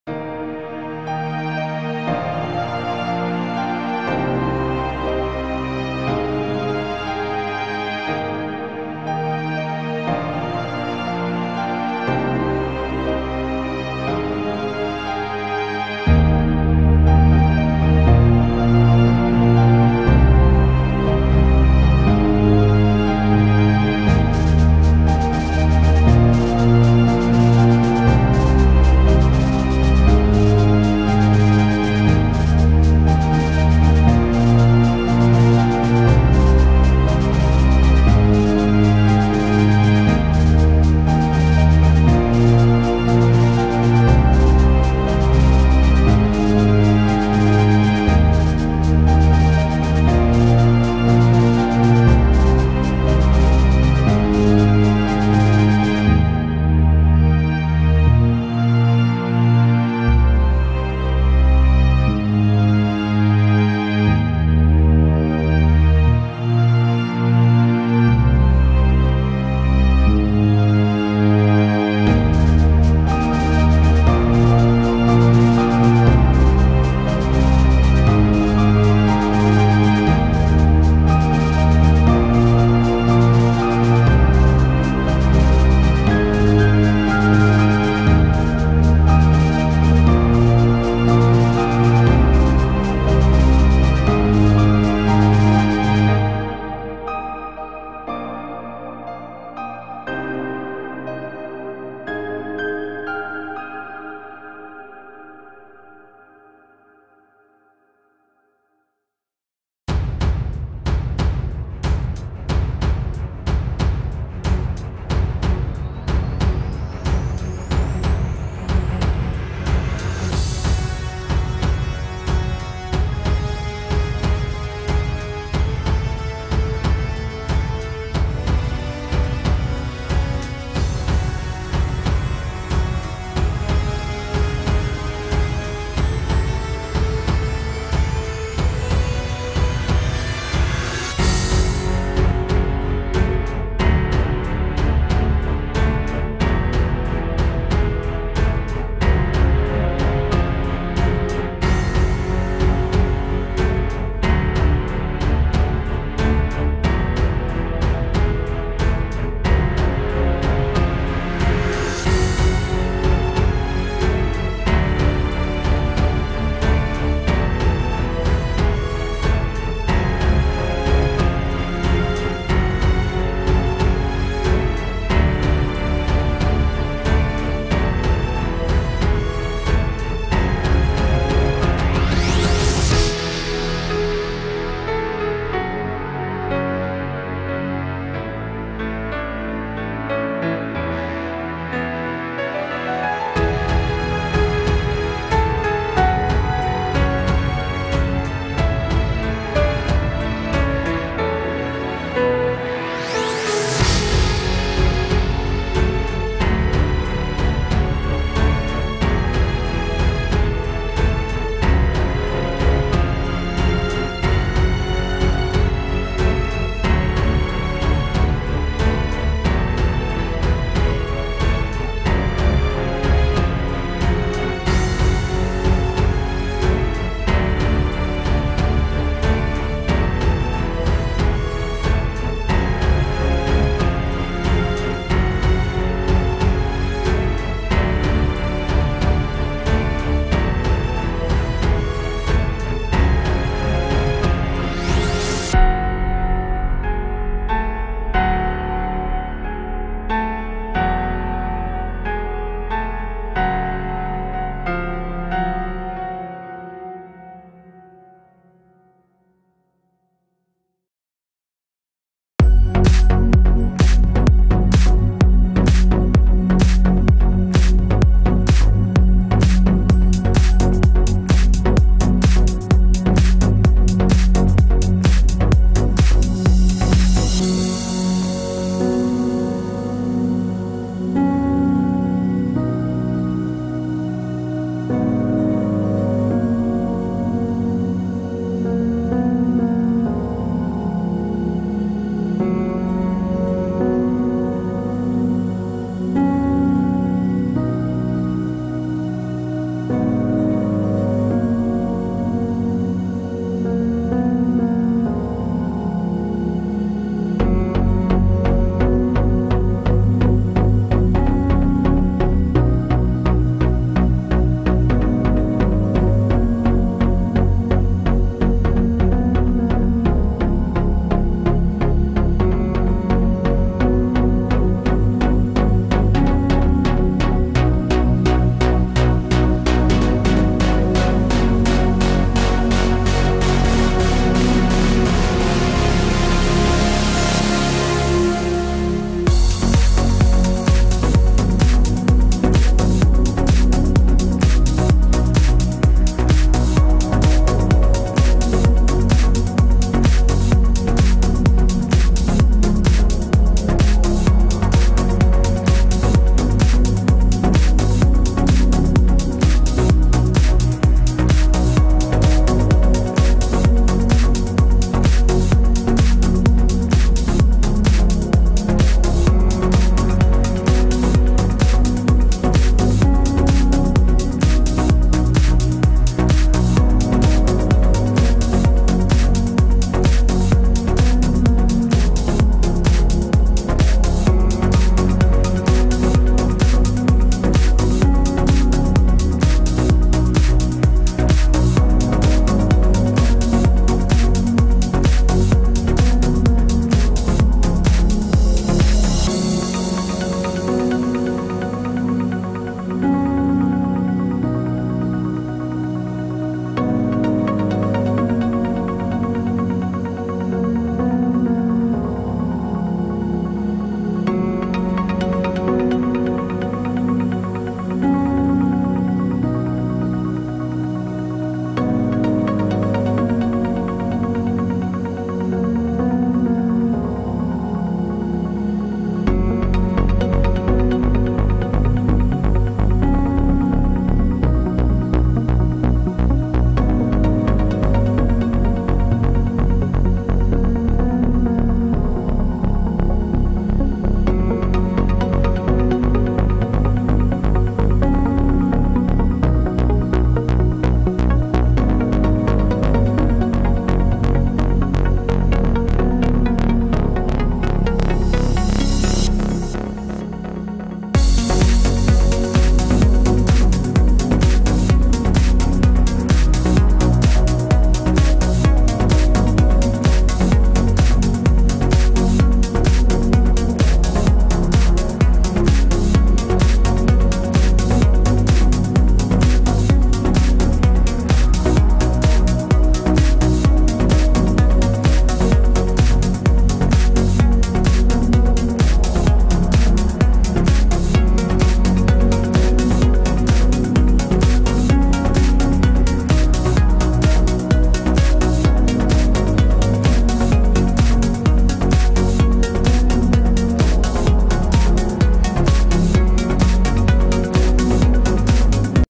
Forex Weekly Technical Analysis for January 15-19, 2024 by Nina Fx
Gold, Silver, EURUSD, GBPUSD, AUDUSD, USDJPY, WTI Oil & Natural Gas technical analysis on H4 chart, for 5 day.